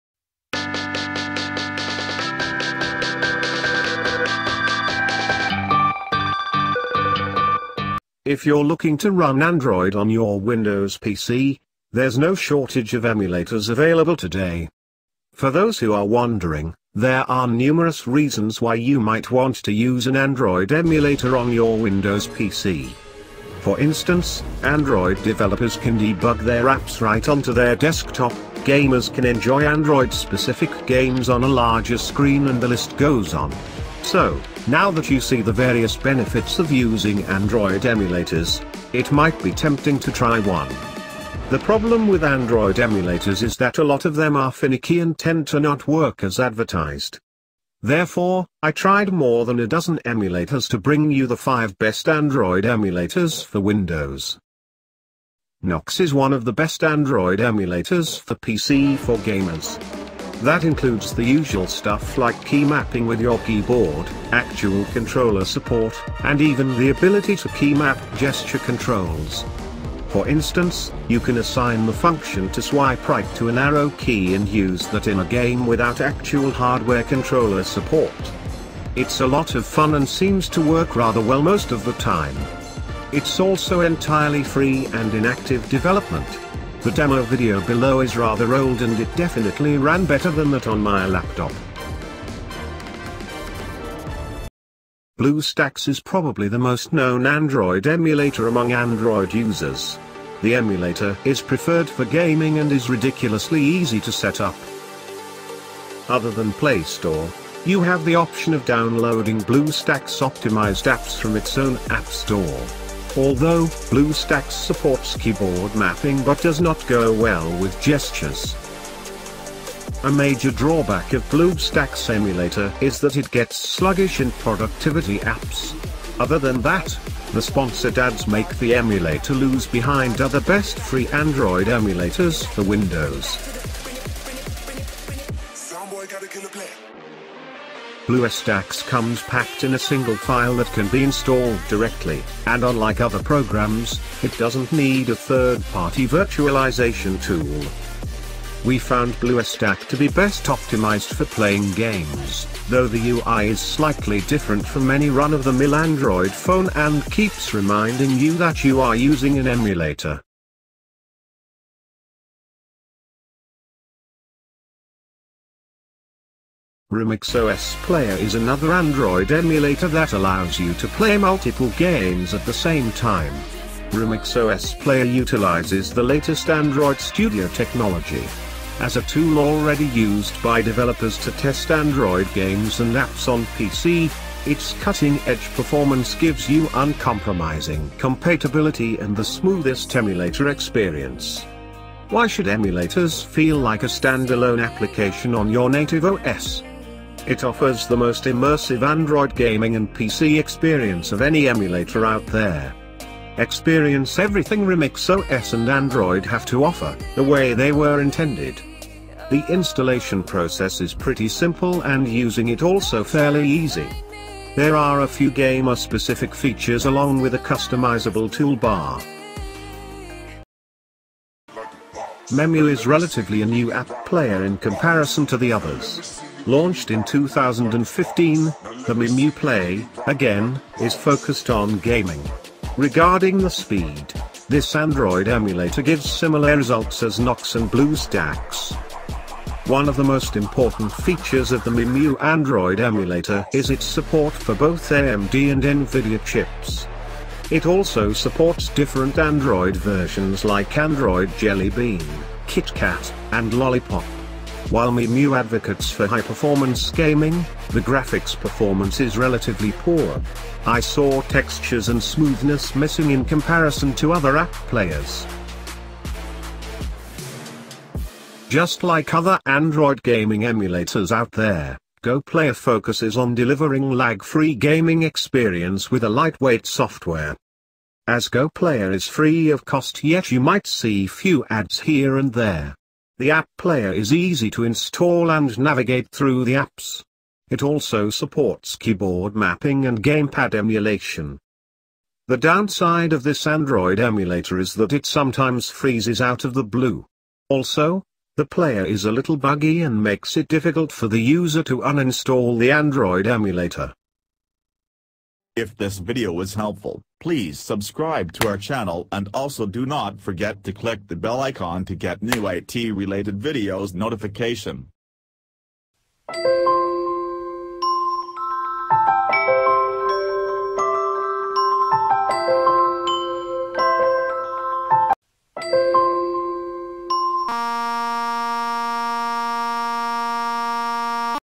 Best 5 Android Emulator For Computer | Android Emulator For PC
Android Emulators are  becoming more popular  now days. Most of the computer user wanted to use Android to their computer. Difference's type of Android software install and games software can use computer easily. So you need Android Emulators download but which emulators best android emulator you need to know before downloading. In this video i will show you top android emulator for pc